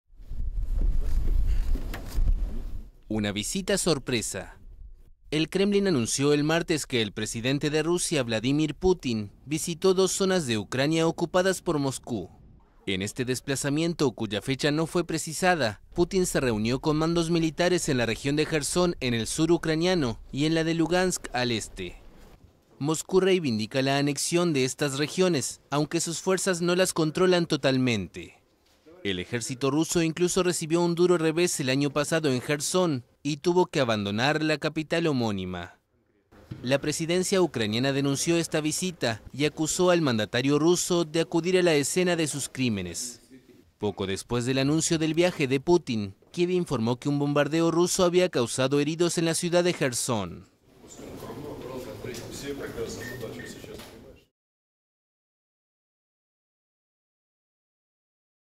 LA GUERRA I Putin visitó regiones ucranianas ocupadas por Rusia
El presidente ruso, Vladimir Putin, realizó una visita sorpresa a dos zonas de Ucrania ocupadas por Rusia, anunció el martes el Kremlin, en momentos en que las fuerzas de Kiev preparan una gran ofensiva.